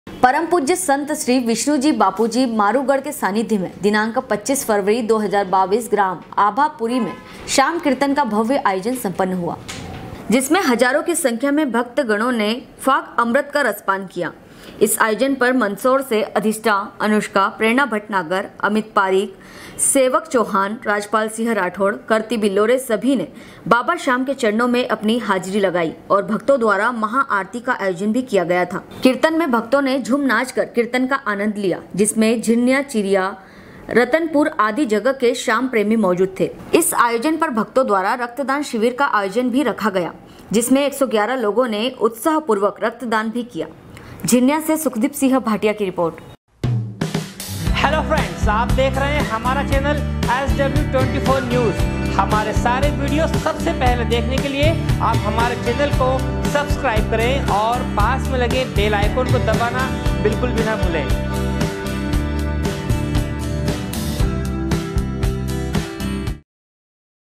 SW 24 News Madhyapradesh
परम पूज्य संत श्री विष्णुजी बापू जी (मारुगड़) के सानिध्य में दिनांक 25 फरवरी 2022 ग्राम आभापुरी में श्याम कीर्तन का भव्य आयोजन सम्पन्न हुआ । झिरन्या से सुखदीप भाटीया की रिर्पोट